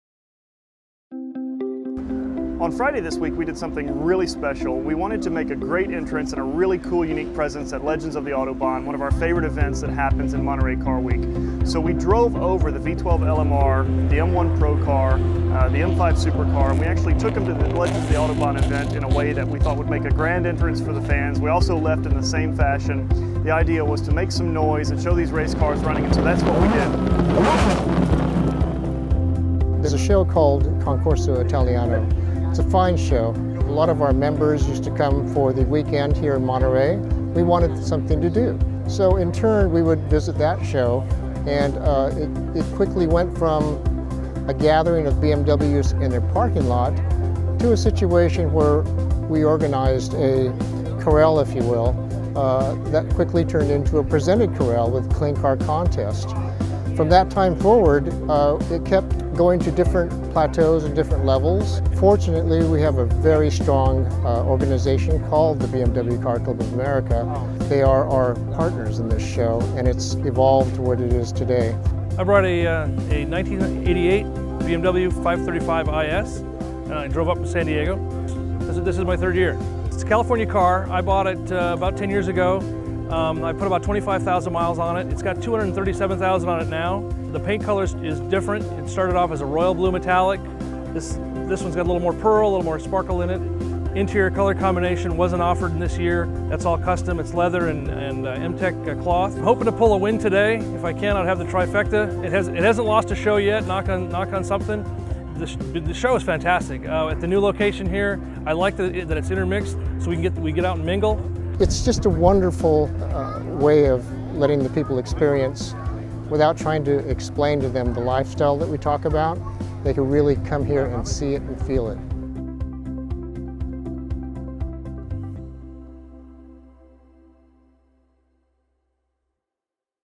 Legends of the Autobahn 2014
Our favorite this year at Pebble Beach was the Legends of the Autobahn venue which has returned for its third-year. What used to be a BMW-focused event, it turned into a German car show with Mercedes-Benz and Porsche joining the bimmer family for the first time in Monterey.

First event took place in 2007 and three years later, the show has grown to 400 cars.